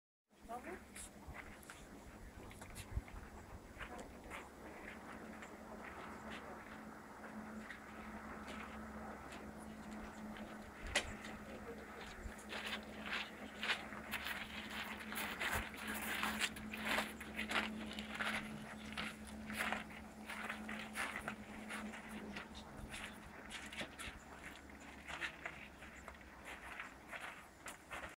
20170814 Croatia Baska view from Sveti Ivan
One of The most Fantastic Place on Earth in my opinion - CROATIA - BAŚKA and view from Sveti Ivan.